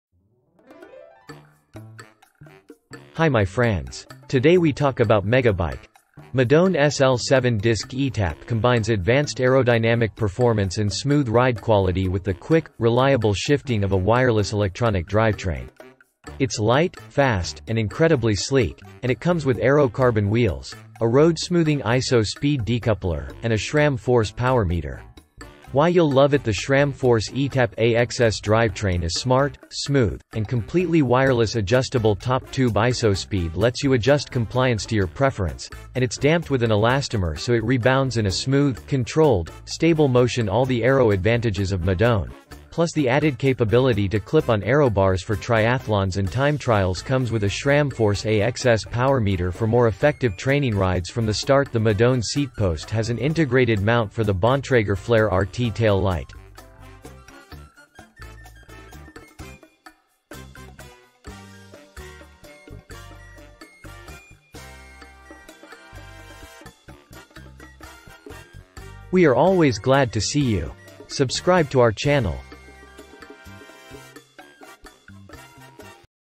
Trek Madone Sl 7 Etap . 2022 New Road Bikes Bike. For Every Season!
"Madone SL 7 Disc eTap combines advanced aerodynamic performance and smooth ride quality with the quick, reliable shifting of a wireless electronic drivetrain. It's light, fast, and incredibly sleek, and it comes with aero carbon wheels, a road-smoothing IsoSpeed decoupler, and a SRAM Force power meter.
The SRAM Force eTap AXS drivetrain is smart, smooth, and completely wireless
Adjustable Top Tube IsoSpeed lets you adjust compliance to your preference, and it's damped with an elastomer so it rebounds in a smooth, controlled, stable motion
All the aero advantages of Madone, plus the added capability to clip on aero bars for triathlons and time trials
Comes with a SRAM Force AXS power meter for more effective training rides from the start
The Madone seatpost has an integrated mount for the Bontrager Flare RT tail light"